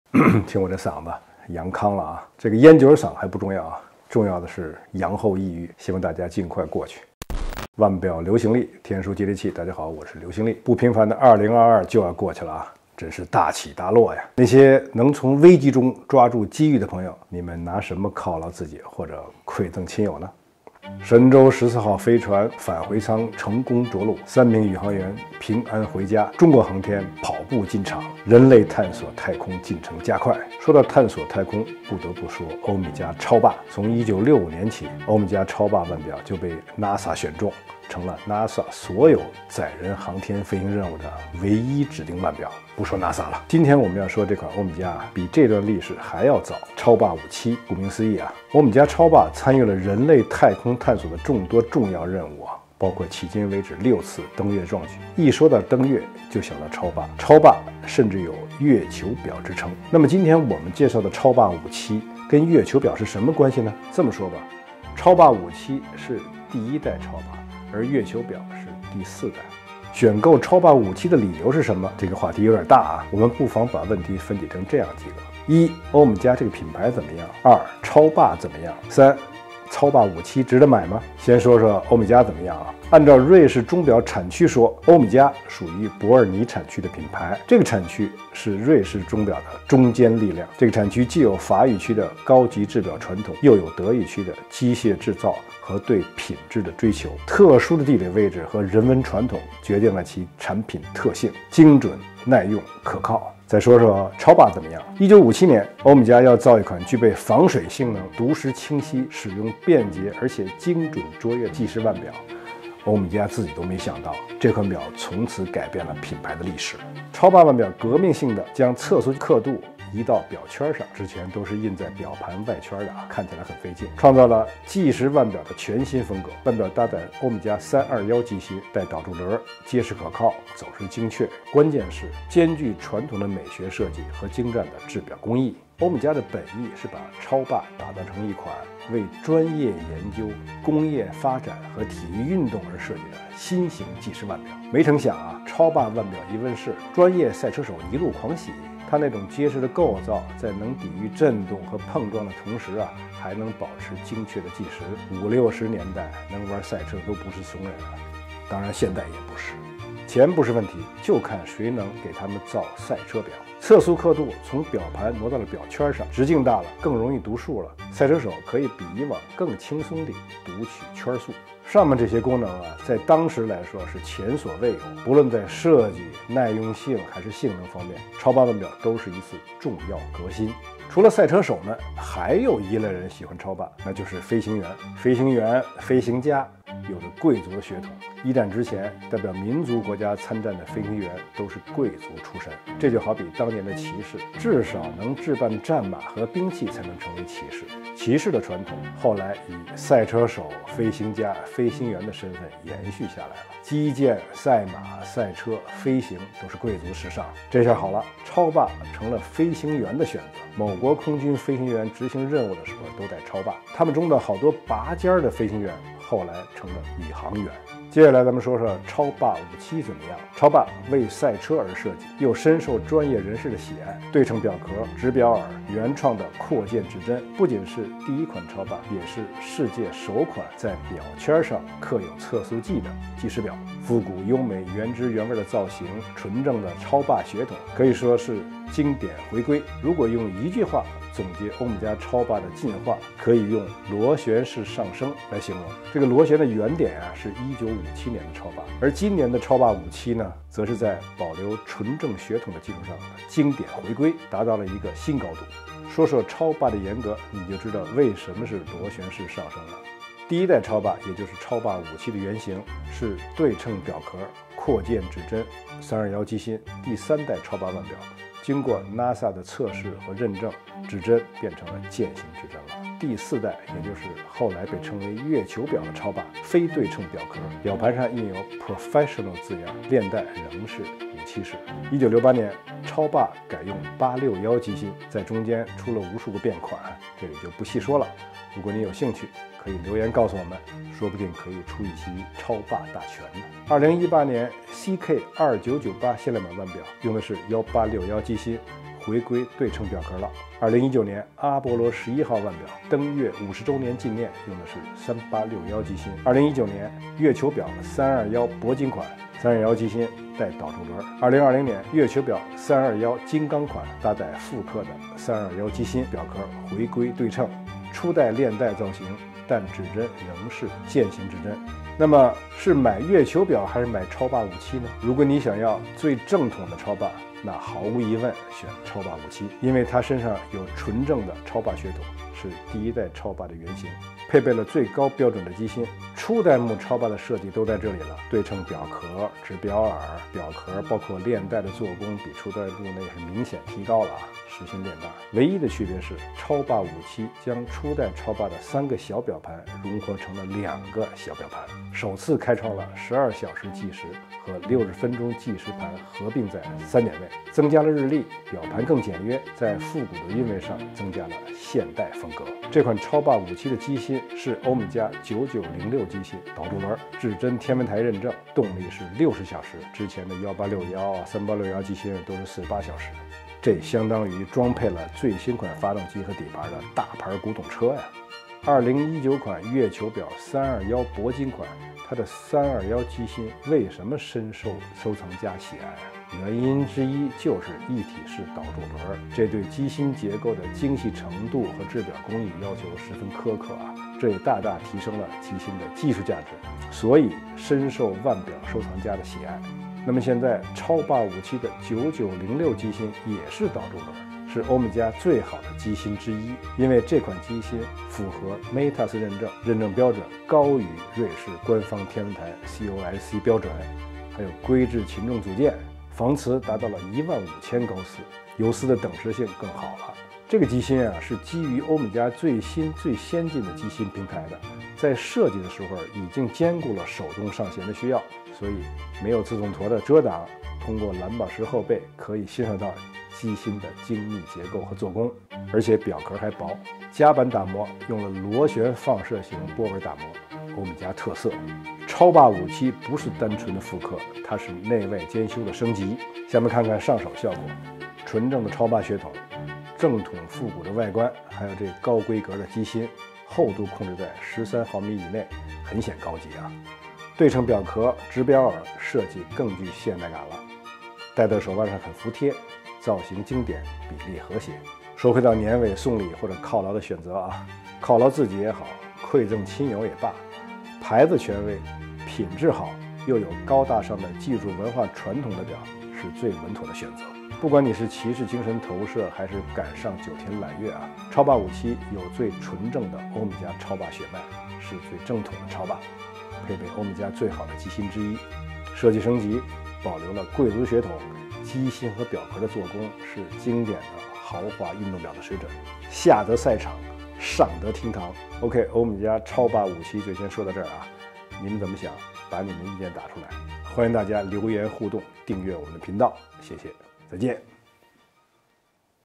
正统超霸 经典回归 上手欧米茄超霸'57
欧米茄著名的超霸’57腕表于2013年首次面世，如今焕新升级，搭载至臻天文台机芯，并以更加纤薄的外观设计荣耀回归。全新腕表尺寸为40.5毫米，推出四个精钢表款，沿袭超霸腕表标志性的传统风格，并以瞩目的全新配色呈现，外观简约经典。